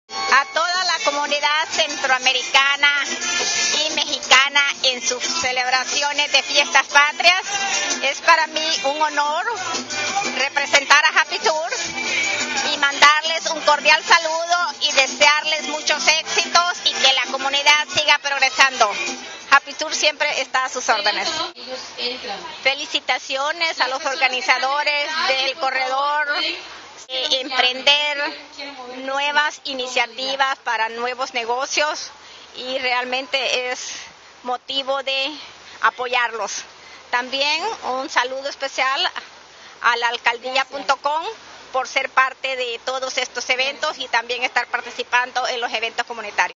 JUANITA VIGIL PROPIETARIA DE HAPPY TOUR - entrevista por: LAalcaldia.com
Si deseas un video gratis para subirlo a Facebook, contáctanos